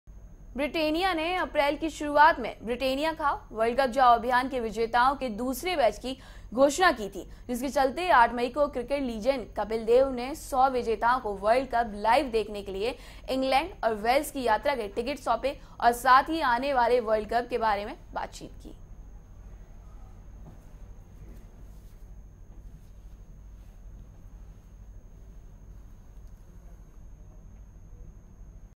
कपिल देव ने 100 विजेताओं को वर्ल्ड कप लाईव देखने के लिए यात्रा के टिकट सौपें.....
ब्रिटेनिया ने अप्रैल की शुरूआत में ब्रिटेनिया खाओ,वर्ल्ड कप जाओ अभियान के विजेताओं के दूसरे बैच की घोषणा की थी. जिसके चलते 8 मई को क्रिकेट लीजेंड कपिल देव ने 100 विजेताओं को वर्ल्ड कप लाईव देखने के लिए इंग्लैण्ड और वेल्स की यात्रा के टिकट सौपें औऱ साथ ही आने वाले वर्ल्ड कप के बारे में बातचीत की...